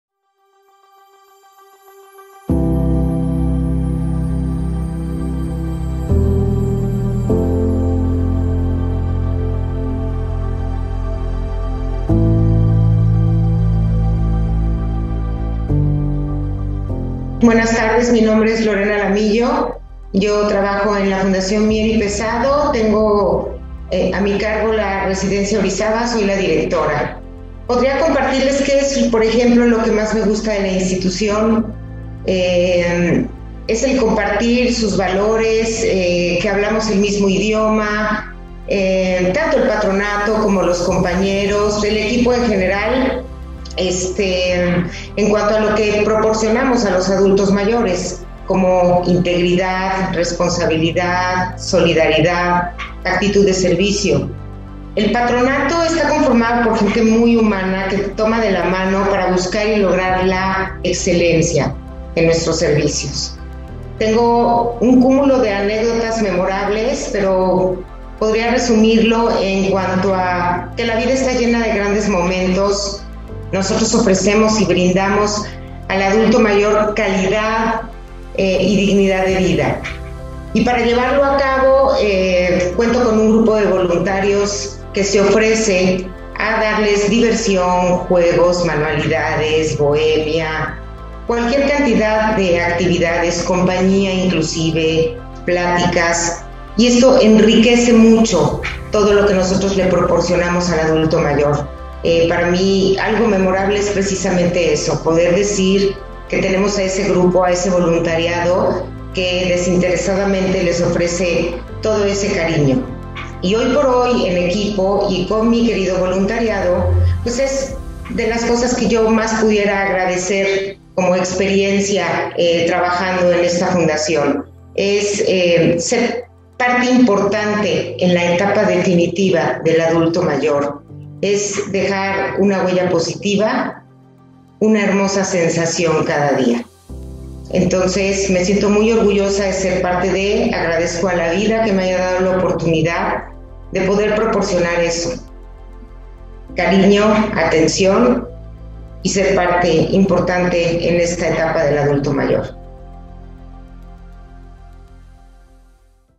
PREMIOS COMPARTIR, XXXIII EDICIÓN I Fundación Mier y Pesado I Testimonio Maria Lorena Alamillo
María Lorena de Jesús Pauler, Directora de la Residencia Orizaba, de la Fundación Mier y Pesado, nos habla sobre su experiencia en la Fundación y los aspectos que más la motivan de su trabajo.

En esta edición de los Premios Compartir, la Fundación Mier y Pesado, I.A.P.,  es la ganadora del Premio a la Institución en Asistencia Social, por el trabajo que realiza para hacer efectivos los derechos de las personas mayores, en  sus residencias en la Ciudad de México y en Orizaba, Veracruz.